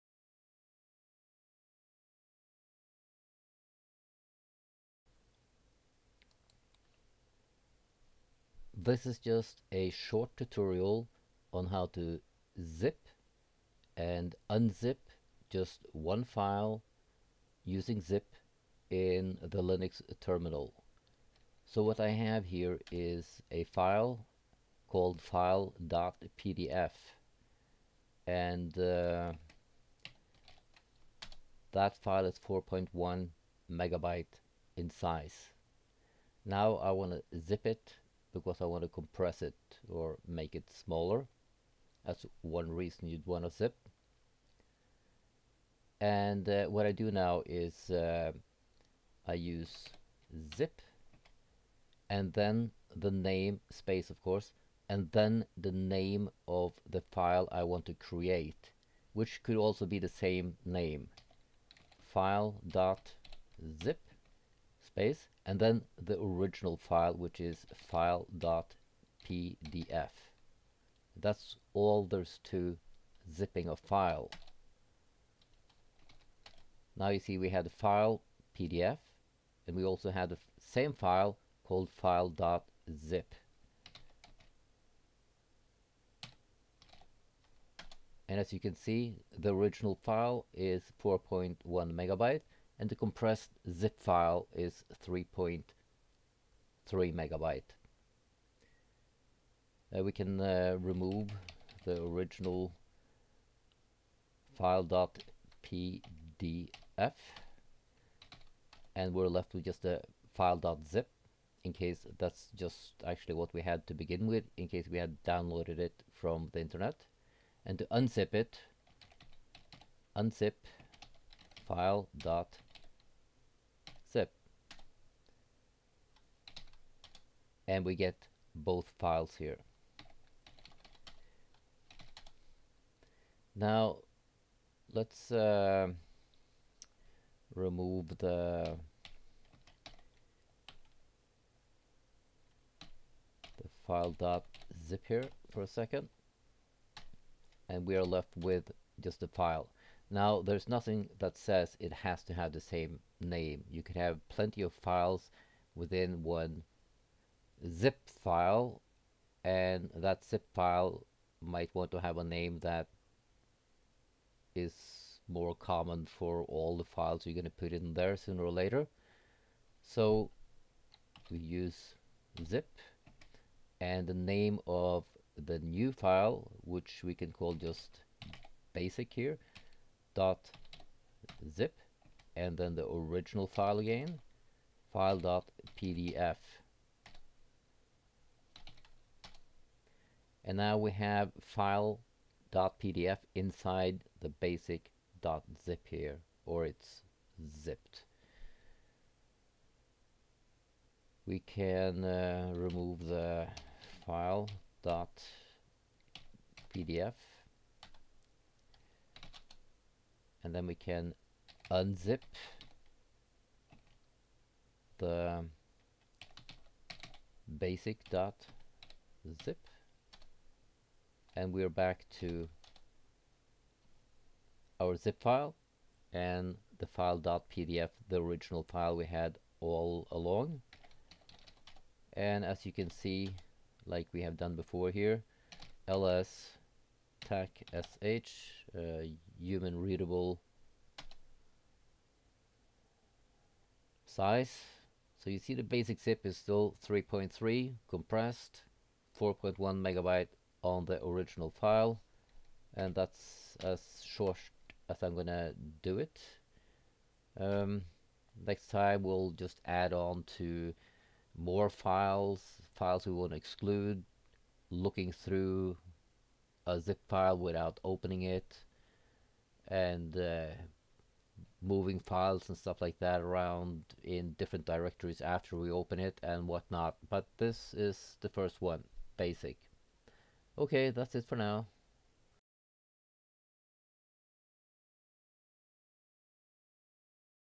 Linux Terminal for Beginners - 1 - zip and unzip a file using zip
Using the terminal in Linux is not common knowledge for everybody. And it seems like we favor a graphical user interface when we have the chance. Put in some fields, knowing Linux and using the terminal is alpha omega. Especially for those aspiring to fields like ethical hacking and information security. I will be covering most of the basic terminal use you need to know. And this tutorial is also a helper for beginners getting their feet wet in Capture The Flag competitions and problem solving. This tutorial is just learning how to zip and unzip one file in the Linux terminal. I am using Windows 10 to Remote Desktop into my Linux Ubuntu 18.04 laptop. Mainly because I haven't found a screen capture program that I like in Linux yet.